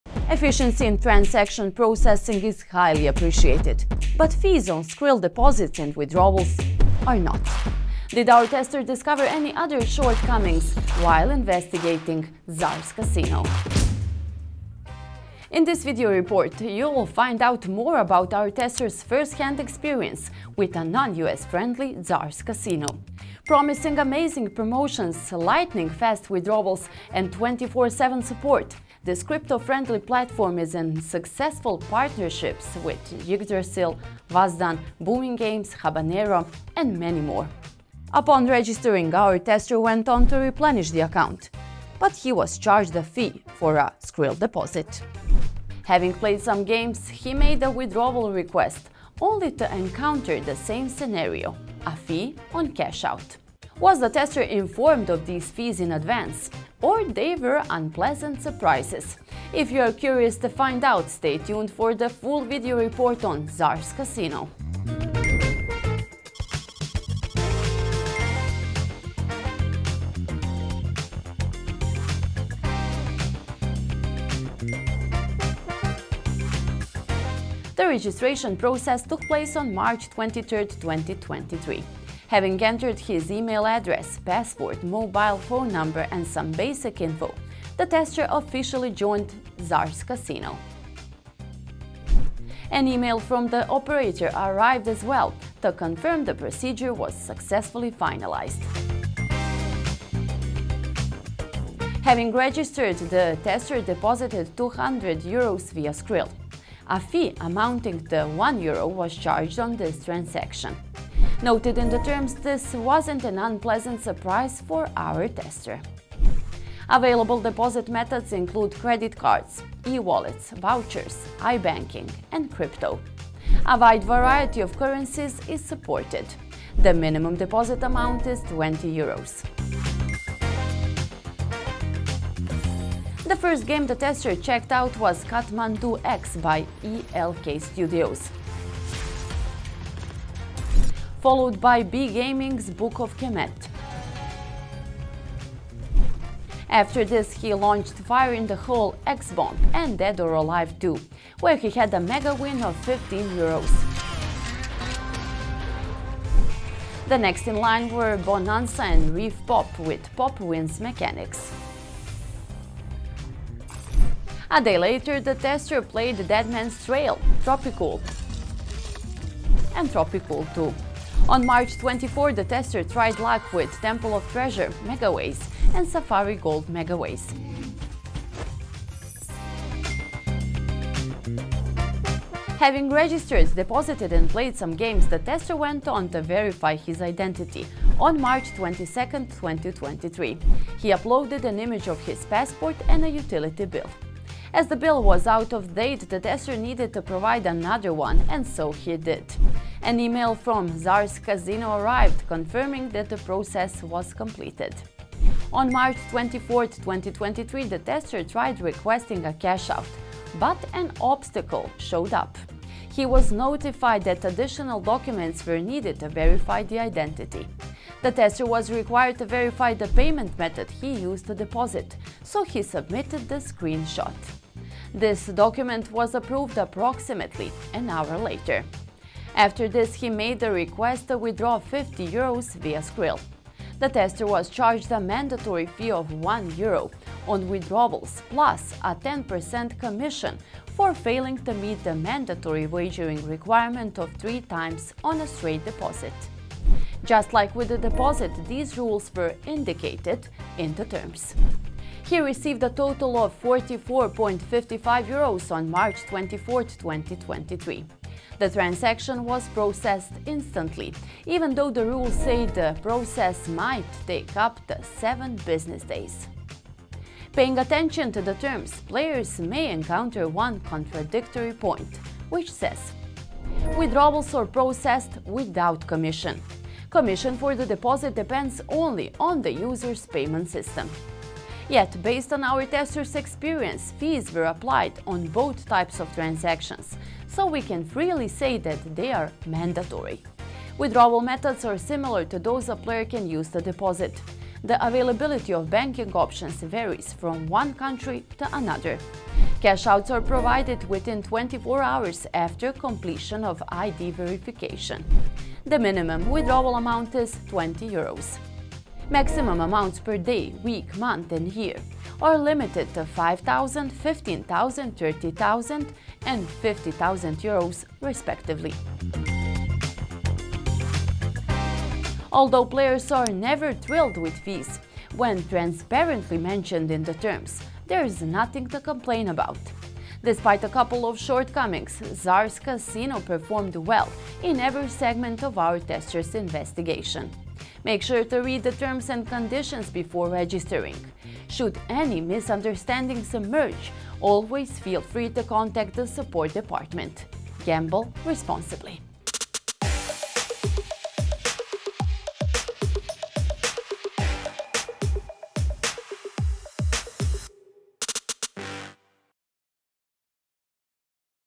Tsars Casino Tested: Handling Mandatory Fees on Skrill Deposit & Withdrawal
Does Curacao-licensed Tsars Casino have what it takes to meet the demands of Bitcoin fans? Eager to find out, our tester registered on this platform and funded the account, which is precisely where he encountered the first obstacle- fee on deposit. Having played some games and submitted documents for ID verification, he requested a cashout. Was he required to pay a fee on this transaction as well? To have the question answered, take time to watch our full video report on Tsars Casino!

00:00 From The Players For The Players
00:15 About Tsars
01:05 Test Rundown
01:15 Registration
01:39 Depositing
02:11 Gameplay
03:00 KYC and Withdrawals
05:21 Conclusion